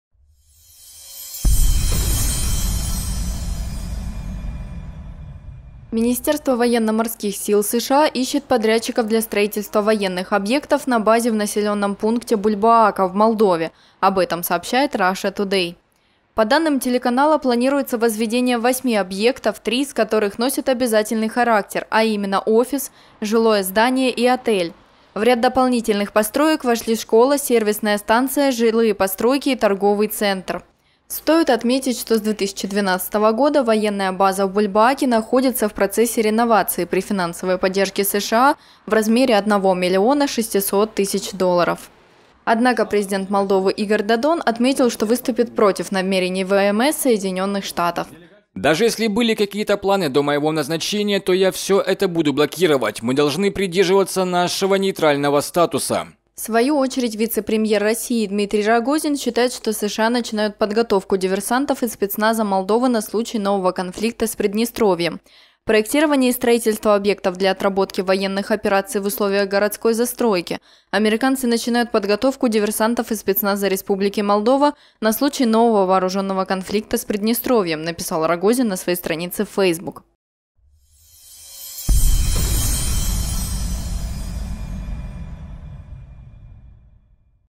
ВМС США хочет построить базу в Молдове
Министерство Военно-морских сил США ищет подрядчиков для строительства военных объектов на базе в населённом пункте Бульбоака в Молдове, об этом сообщает RT
По данным телеканала, планируется возведение восьми объектов, три из которых носят обязательный характер, а именно - офис, жилое здание и отель. В ряд дополнительных построек вошли школа, сервисная станция, жилые постройки и торговый центр.
Стоит отметить, что с 2012 года военная база в Бульбоаке находится в процессе реновации при финансовой поддержке США в размере $1,6 млн.
Однако президент Молдовы Игорь Додон отметил, что выступит против намерений ВМС Соединённых Штатов.
«Даже если были какие-то планы до моего назначения, то я всё это буду блокировать. Мы должны придерживаться нашего нейтрального статуса»
В свою очередь вице-премьер России Дмитрий Рогозин считает, что США начинают подготовку диверсантов и спецназа Молдовы на случай нового конфликта с Приднестровьем.
«Проектирование и строительство объектов для отработки военных операций в условиях городской застройки" - американцы начинают подготовку диверсантов и спецназа Республики Молдова на случай нового вооруженного конфликта с Приднестровьем», - написал Рогозин на своей странице в Facebook.